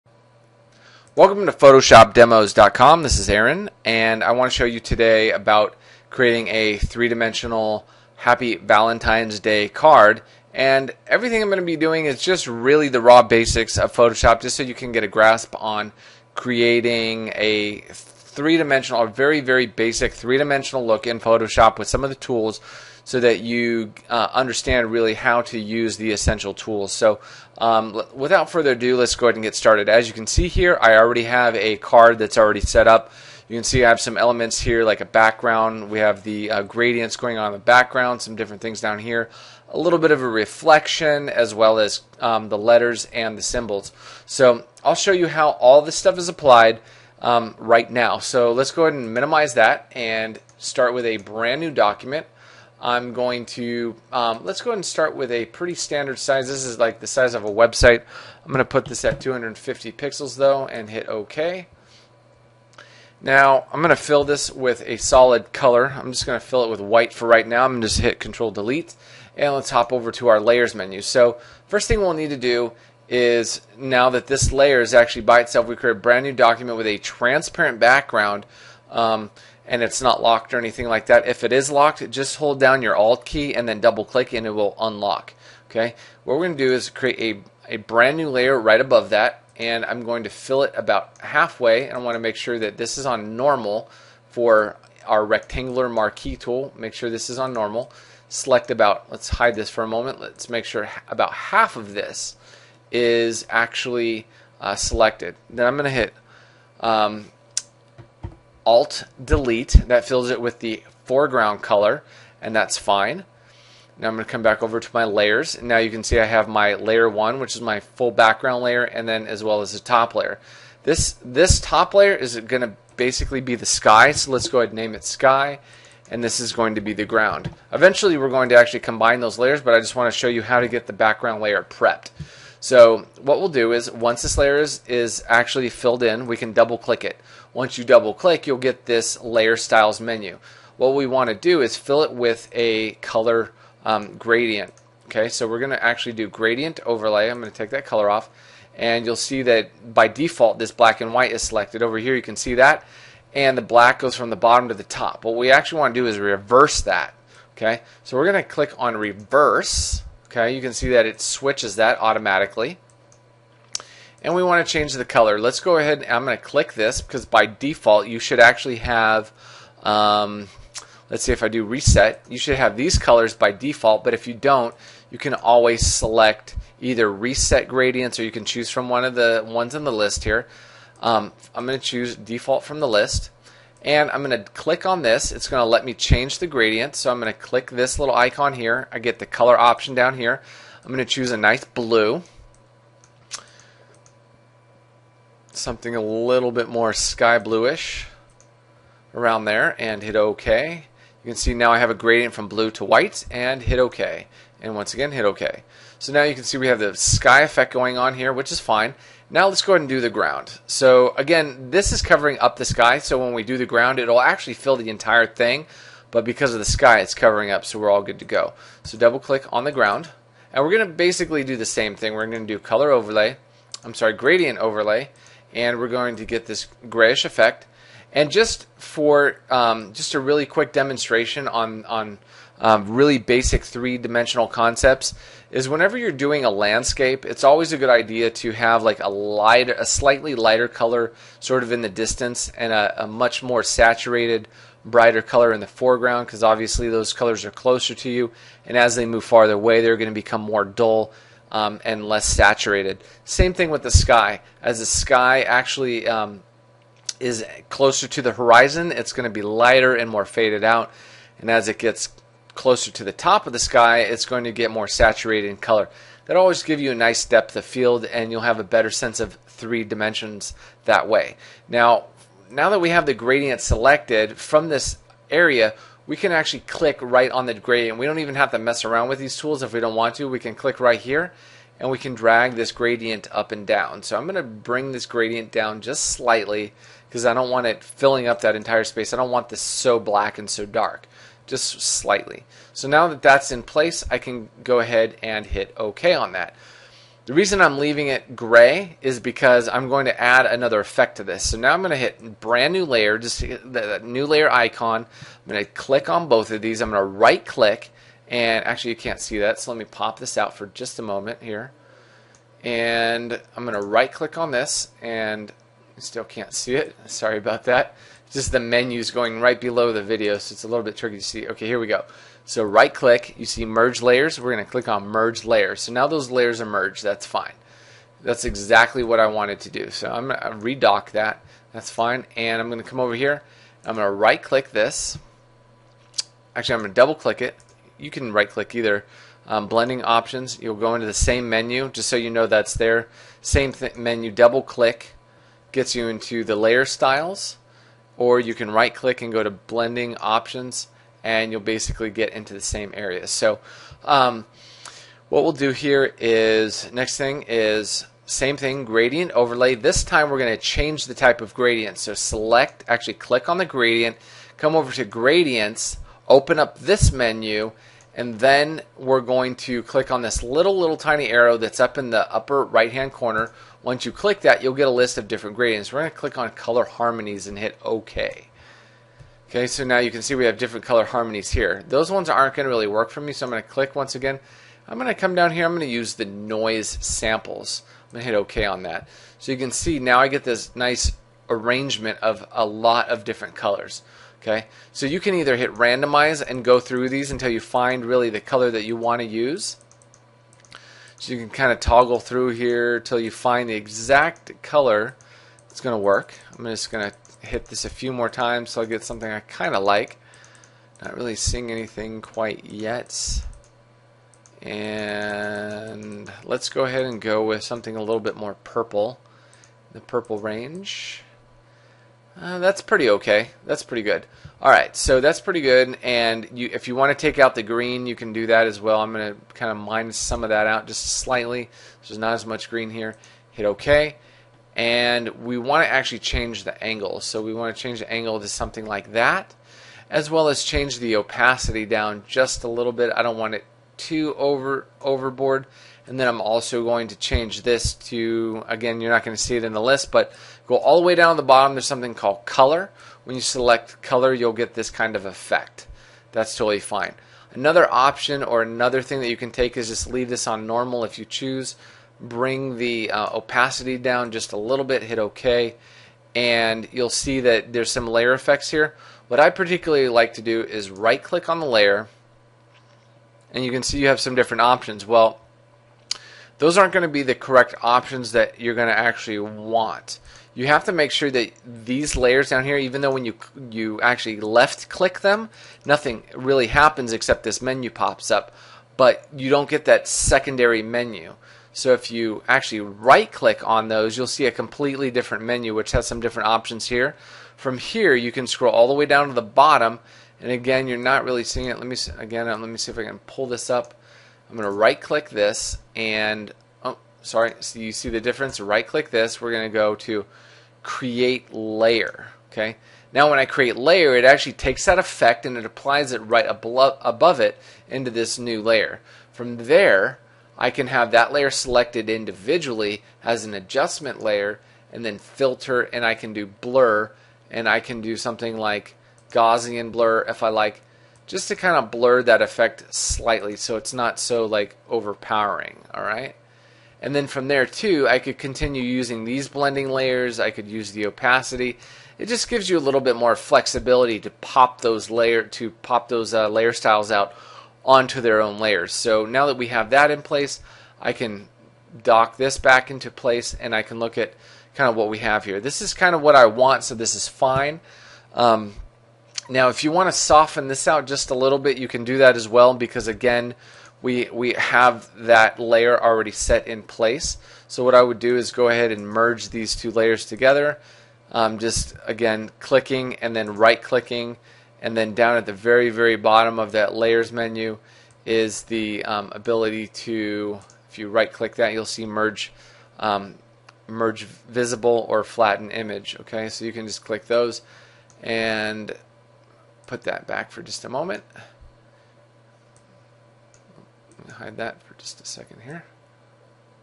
Valentines Card in Photoshop 1 of 3
The cheesiest way to create a valentine's card in Photoshop. A basic demonstration of creating a 3D Card in photoshop. Step by step video to see exactly how to make a semi-realistic Valentines day card.